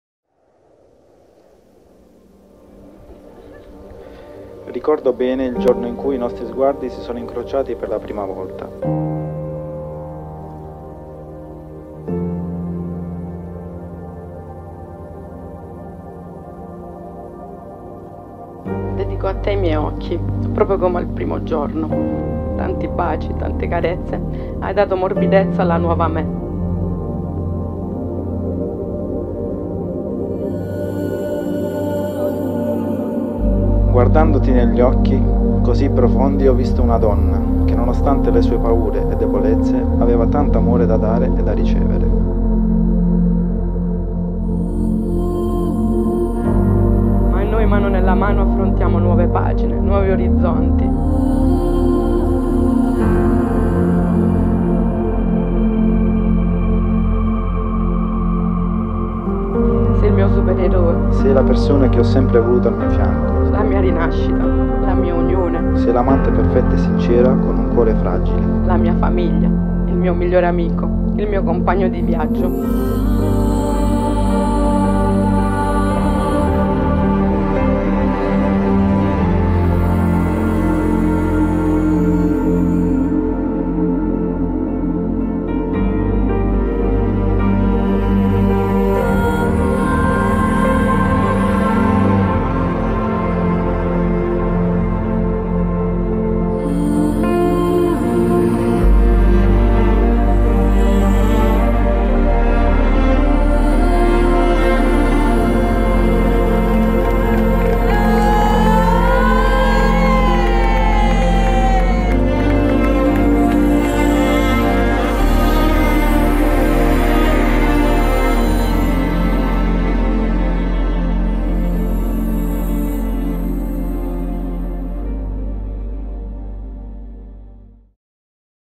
Antonietta e Fabio scelgono Sinfonie di Nozze per un rito civile autentico e significativo
RITO CIVILE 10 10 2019

CEREMONY PLANNING
Sinfonie di Nozze

MASSERIA TORRE PIETRA
MONOPOLI (BA)